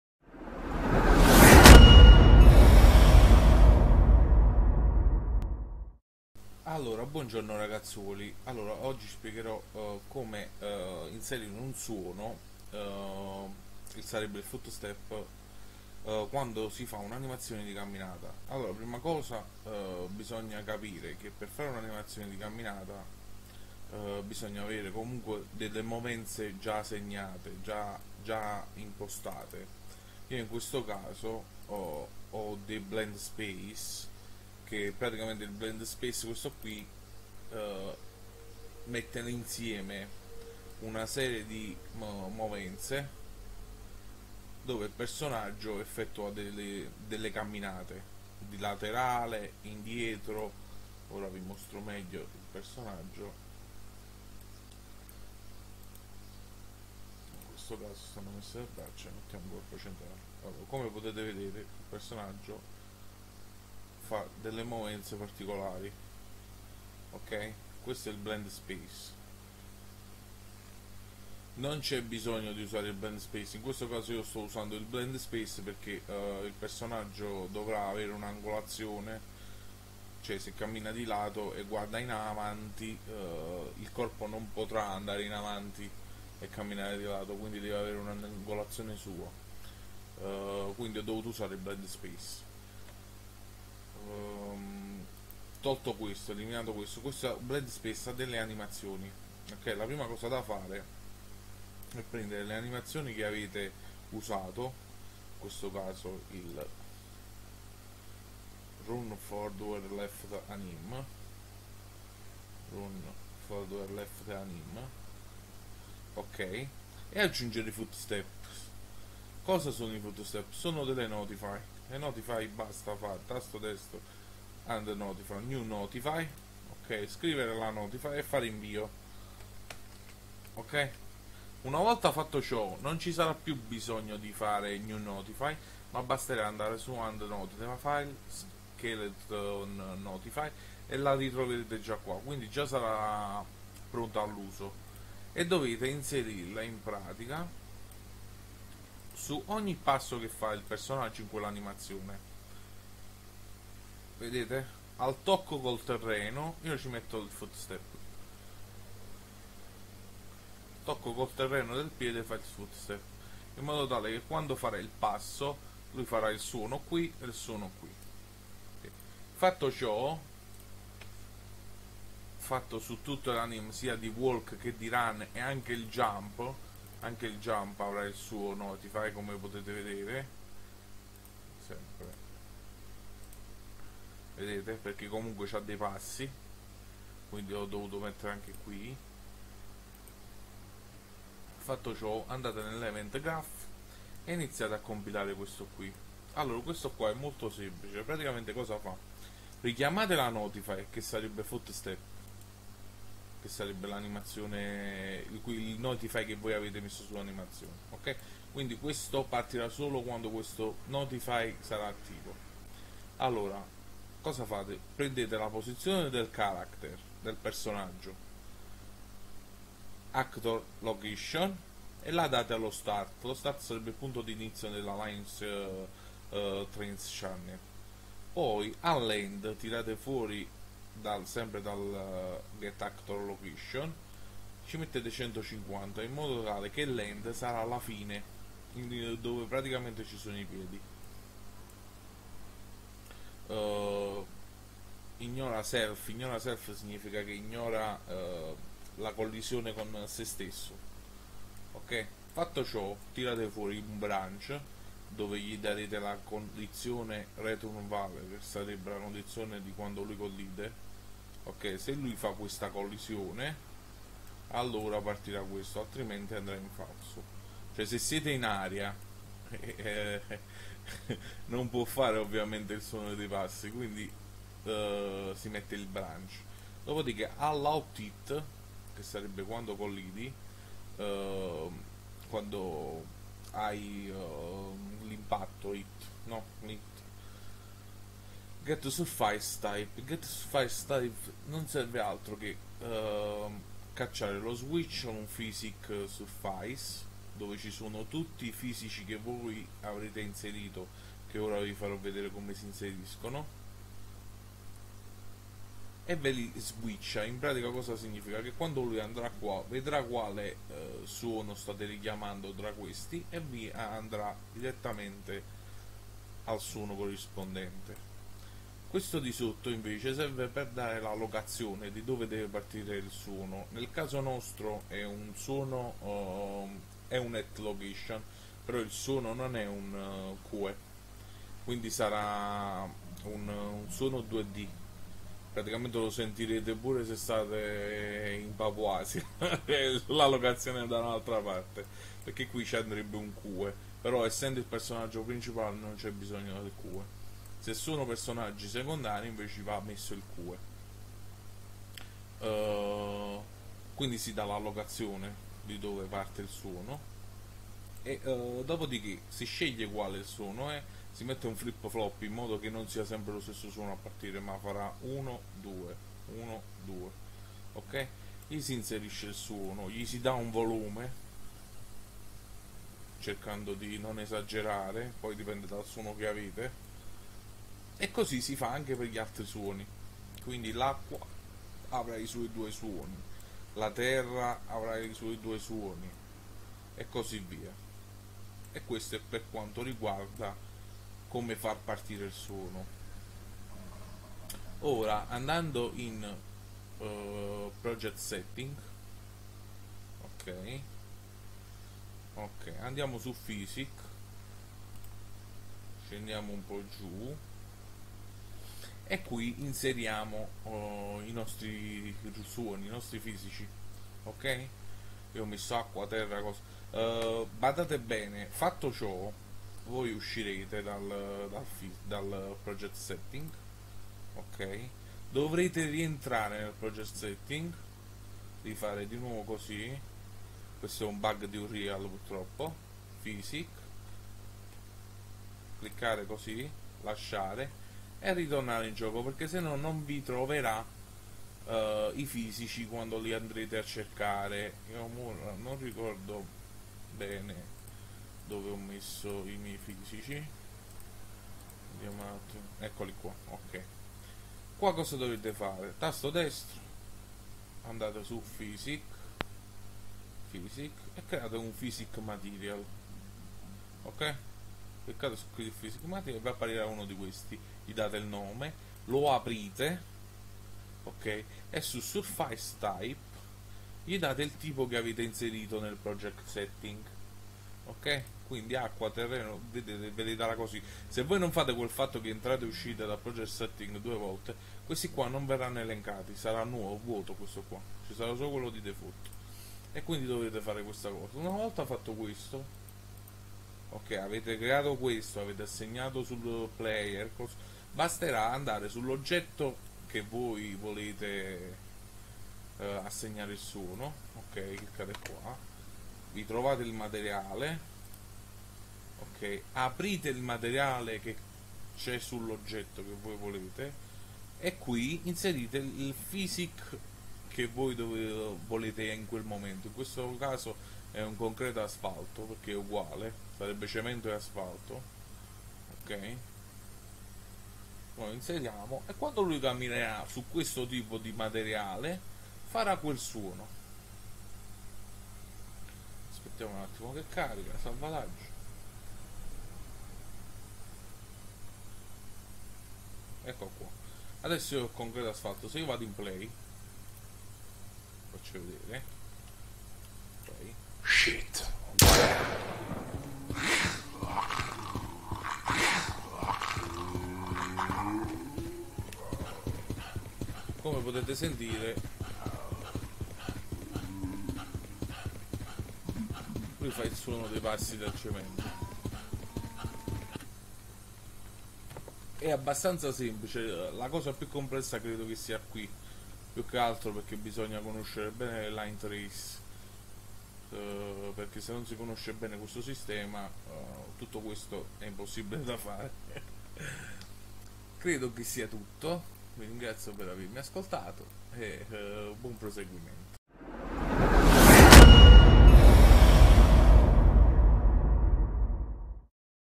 tutorial unreal engine 4 foot step suono passi ITA
come inserire il suono di passi in ue4